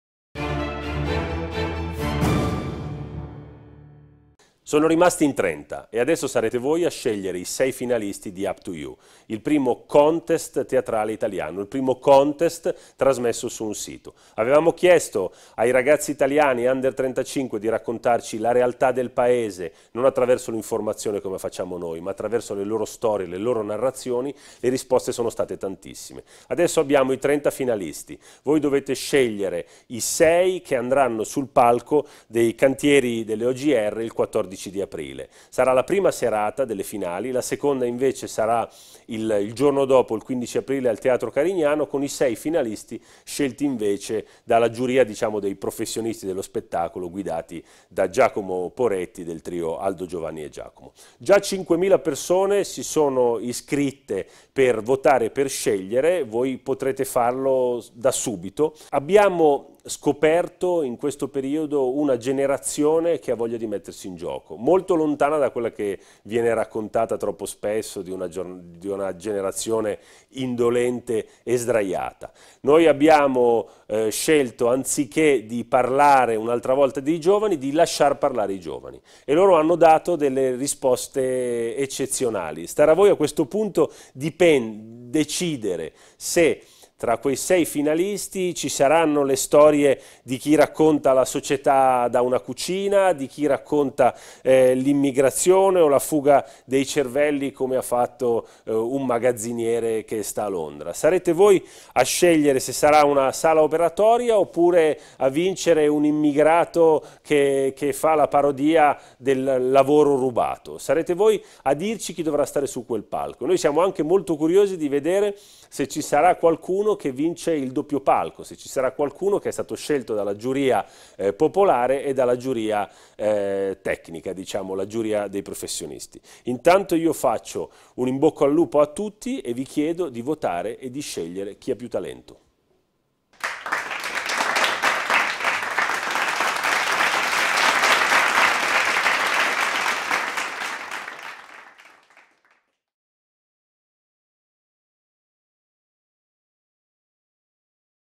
L'invito del Direttore: votate e prenotatevi, it's UP2U!
Il Direttore de La Stampa Mario Calabresi invita tutti voi, che ancora non avete votato, a registrarvi sul sito di UP2U! per votare entro l'8 aprile i sei finalisti che si aggiudicheranno il palco dei Cantieri OGR il 14 aprile alle ore 21, quando lo spettacolo di UP2U! avrà inizio.

Ma non solo: siete tutti invitati a prenotare il vostro posto gratuito alle due serate finali che si svolgeranno a Torino sui palchi più prestigiosi della città.

Saranno i  grandi nomi del giornale a presentare le due serate finali: il 14 aprile, sul palco dei Cantieri OGR, Luca Dondoni e il Vicedirettore Massimo Gramellini vi faranno conoscere i vincitori decretati dalla giuria popolare, mentre il 15 aprile,  al Teatro Carignano, toccherà invece a Giacomo Poretti e al Direttore Mario Calabresi dirigere lo show dei vincitori della giuria tecnica.

Noi abbiamo scoperto una generazione sogna, che ha talento e molto da dire, che si mette in gioco ed è disposta ad attraversare l'Italia per far sentire la propria voce sul palco.
A questo punto tocca a voi conoscere la generazione UP2U!

Votate e prenotatevi: non perdeteveli, sul palco vedrete talenti, non personaggi da talent.

Per informazioni: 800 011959 dal lunedì al venerdì 9.30/14.30